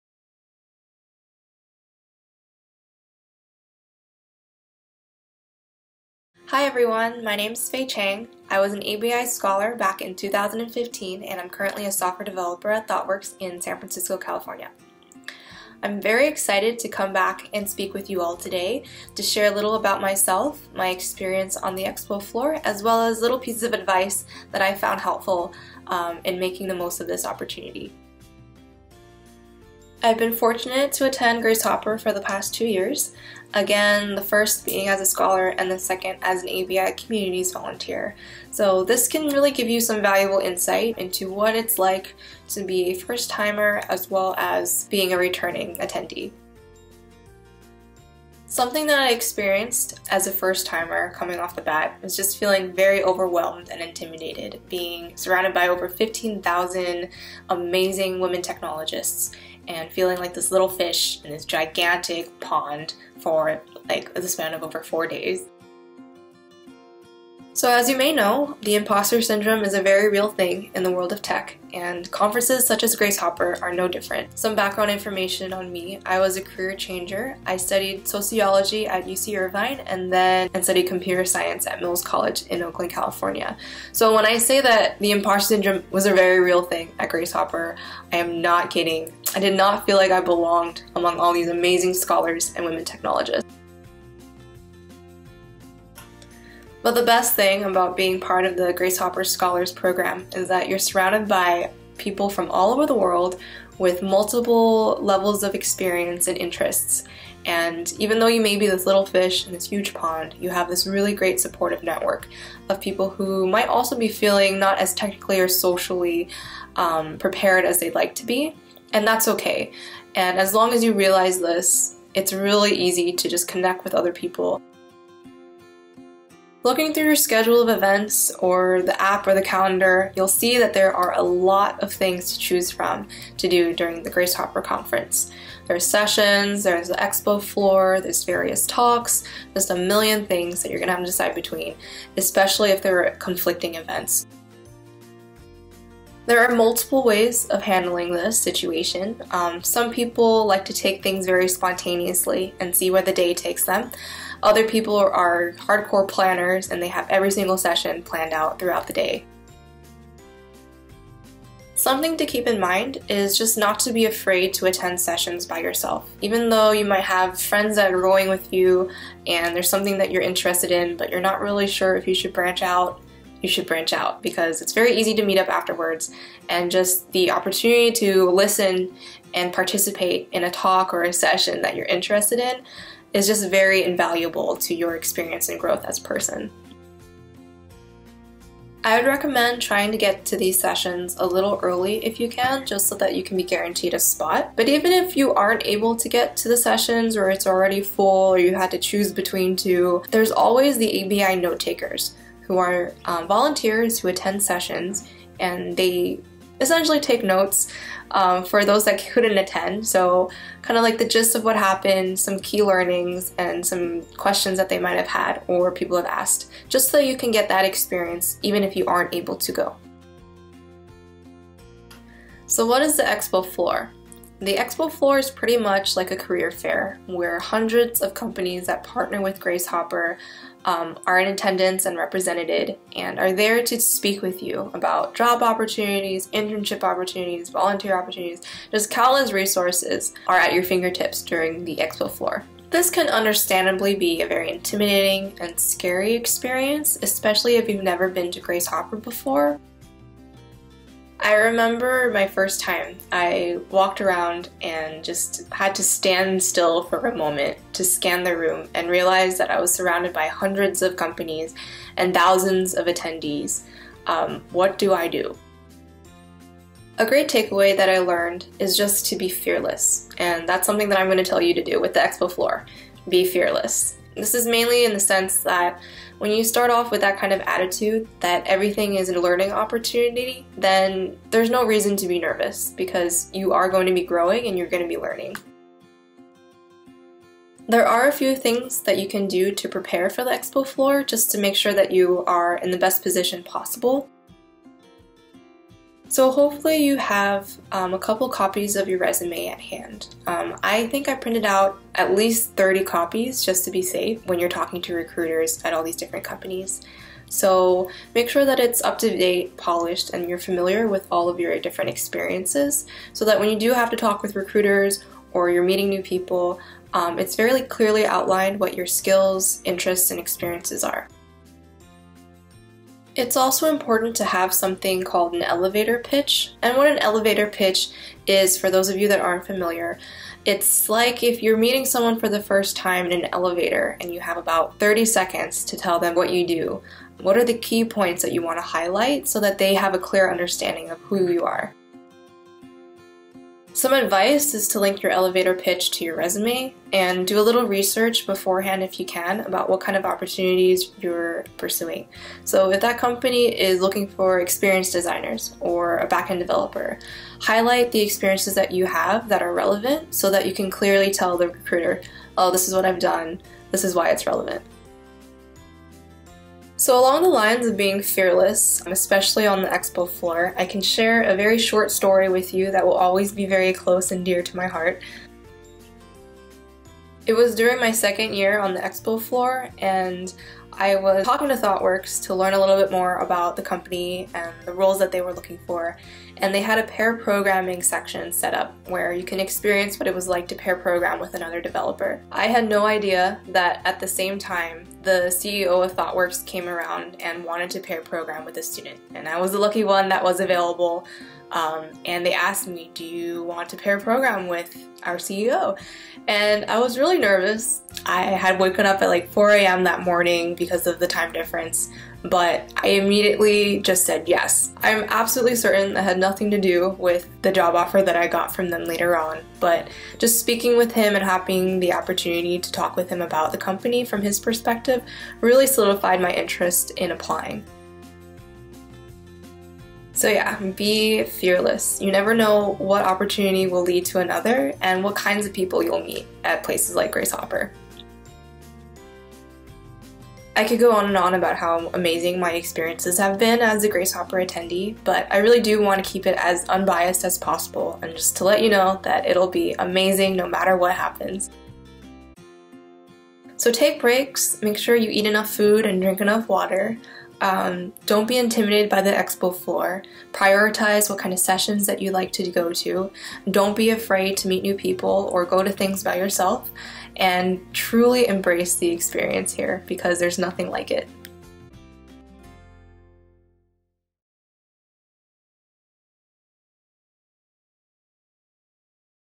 Faye Cheng, AnitaB.org Scholar, Shares Tips
Faye Cheng, a 2015 AnitaB.org GHC Scholar, shares her experience at the Grace Hopper Celebration. She offers tips and suggestions for scholars to get the most out of their time at the celebration.